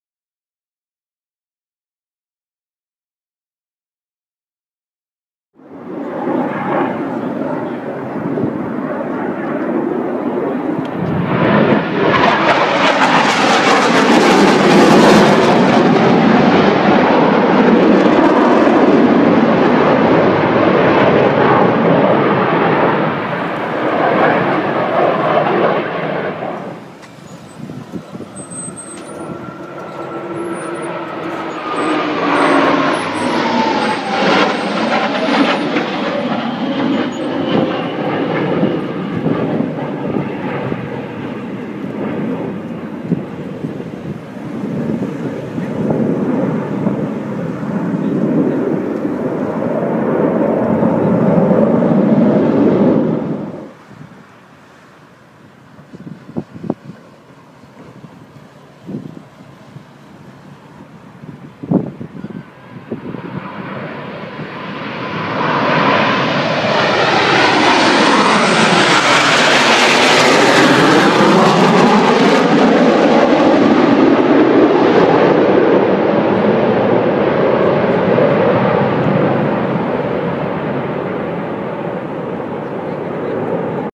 Blue Angels
When the squadron receives a F/A-18 Hornet from the fleet, which are at the end of their carrier arrestment functionality, we make a variety of modifications, including removing the nose cannon to install a smoke-fluid system, inverting a fuel pump, installing a stop watch and adjustable constant-tension stick spring, as well as the world-wide recognizable paint scheme.